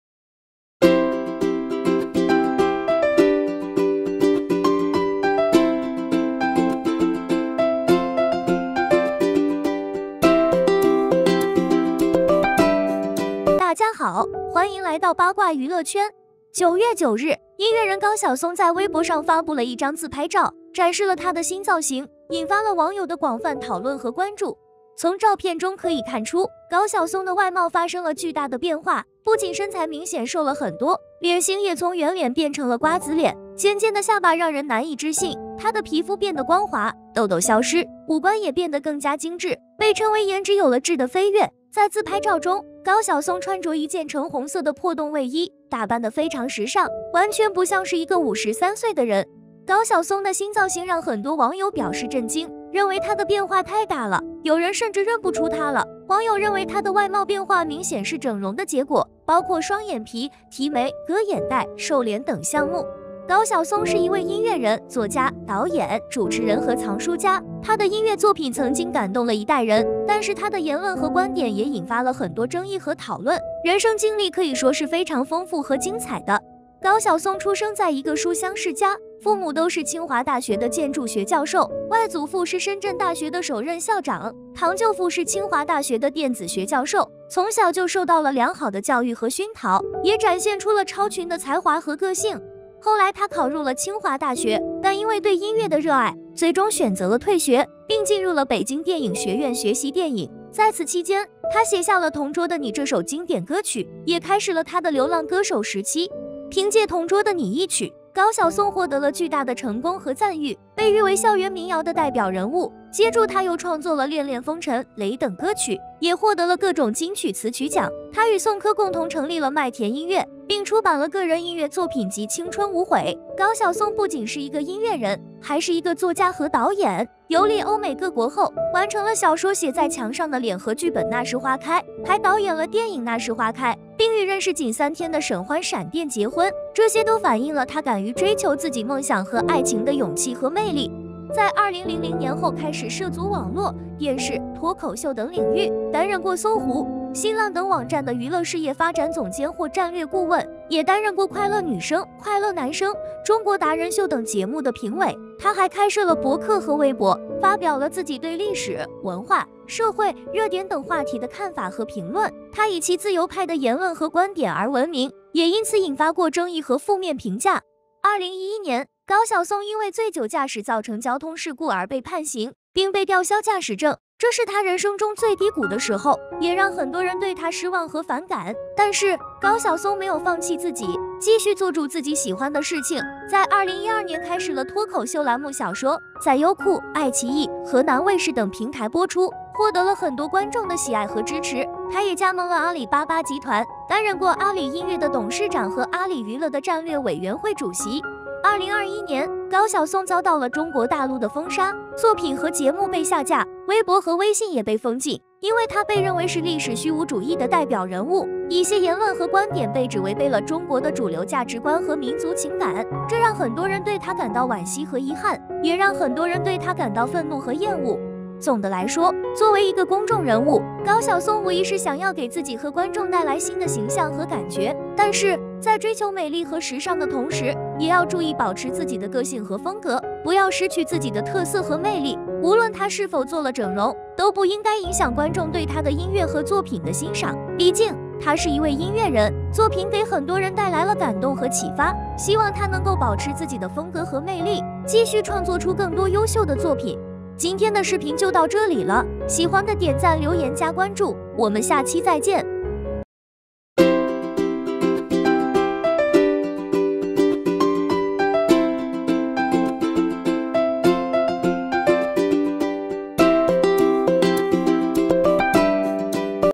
高曉松曬近照暴瘦認不出，疑過度醫美撞臉華晨宇，卡姿蘭大眼瓜子臉宣告正式復出！
高曉松曬近照暴瘦認不出，疑過度醫美撞臉華晨宇，卡姿蘭大眼瓜子臉宣告正式復出！高曉松